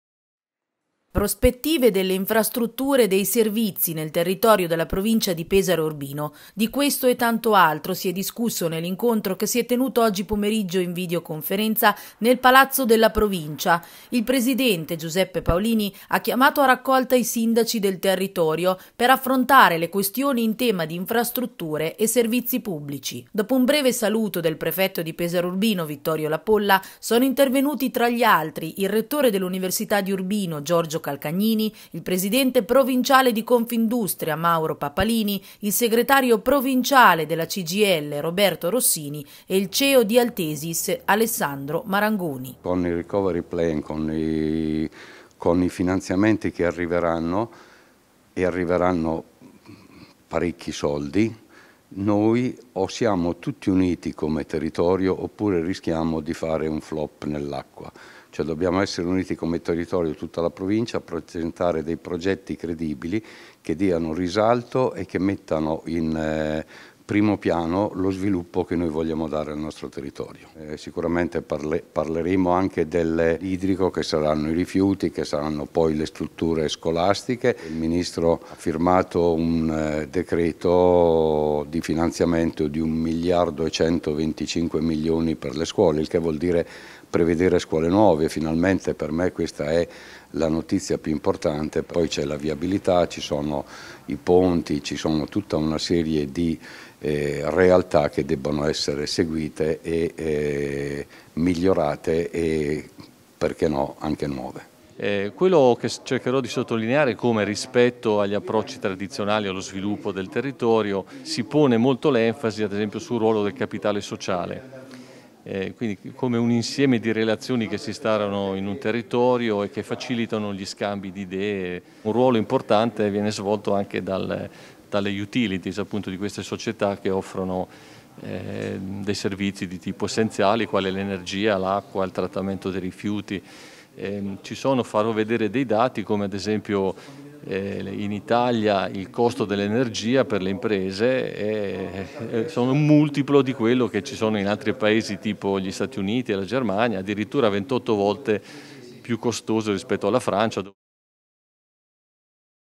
infrastrutture e servizi pubblici, summit in provincia
La Provincia di Pesaro e Urbino ha chiamato a raccolta i sindaci per un confronto sullo sviluppo dei servizi e delle infrastrutture.